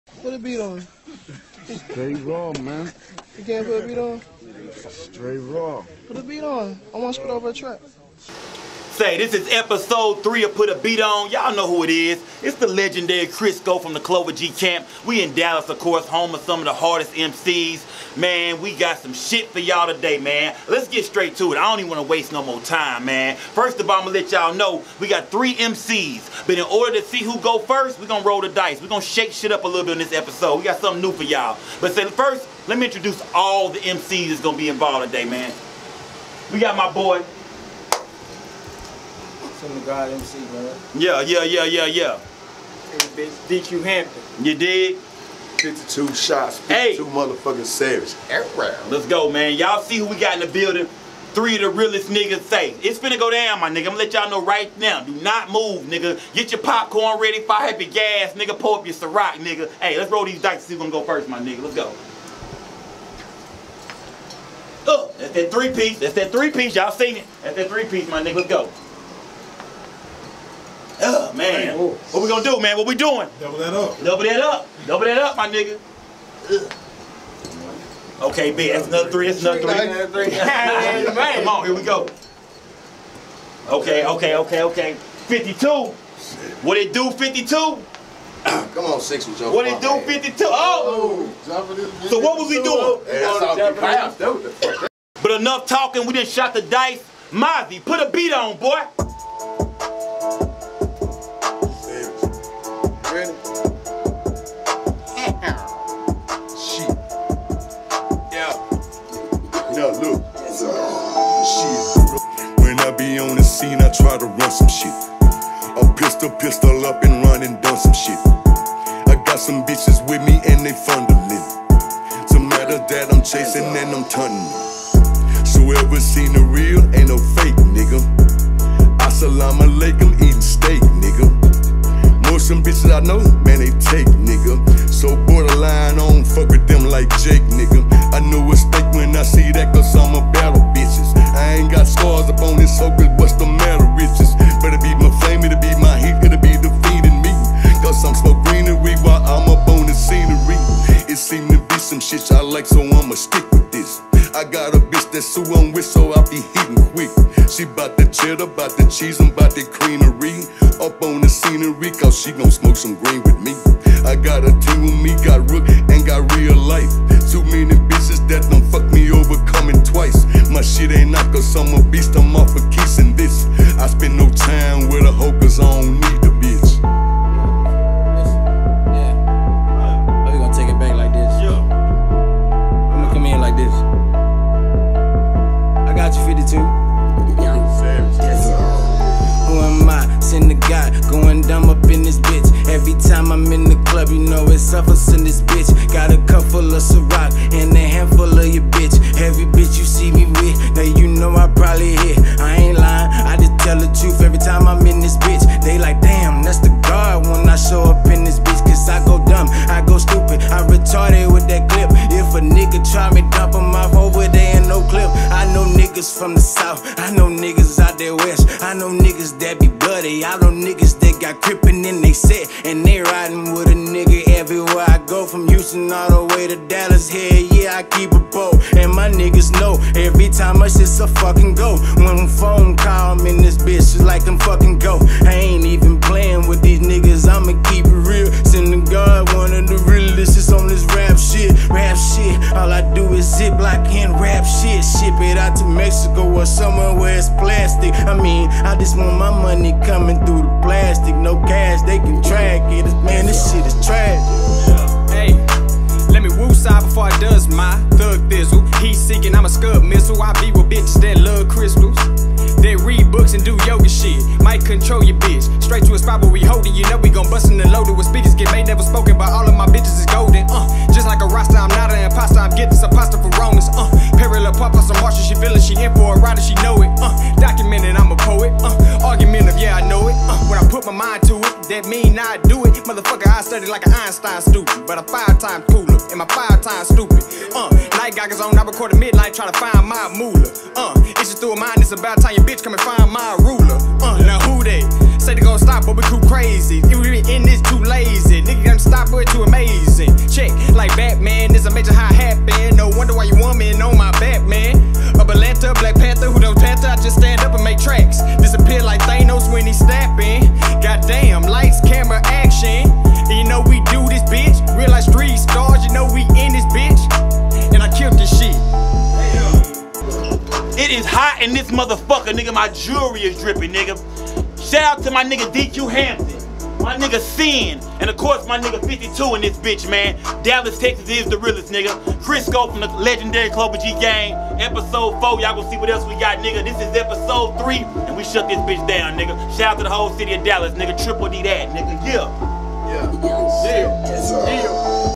PutABeatOn Episode 3 | 52 Savage x DQ Hampton x Sin Da God MC | Hosted by Chris Sko
1st: @52Savage
2nd: @SinDaGodMC
3rd: @dq254tx